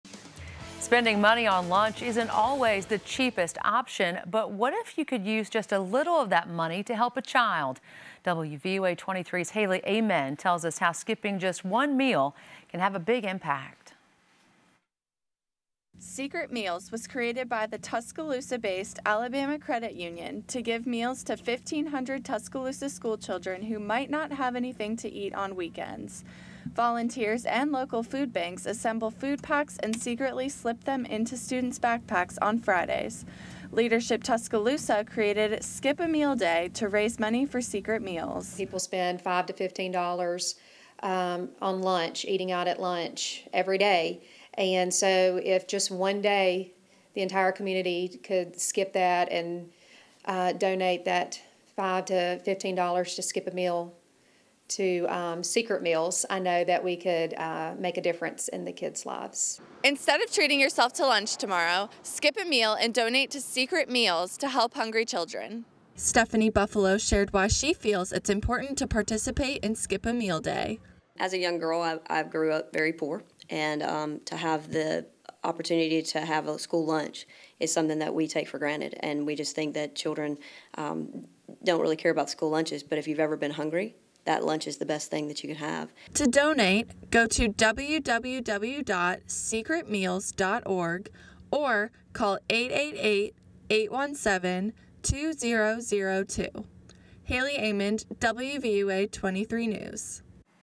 SECRET MEALS ASKS YOU TO SKIP A MEAL SO CHILDREN WON'T
Leadership Tuscaloosa wants you skipping lunch Wednesday, but it's for a great cause.

Skip a Meal Day is a fundraiser for Secret Meals for Hungry Children, asking that instead of buying lunch Wednesday, you donate the amount you'd have paid to the organization.

Secret Meals helps children who rely on free or reduced breakfasts and lunches served in public schools. Food packs are slipped into students' backpacks on Fridays so they've got plenty of food over the weekend.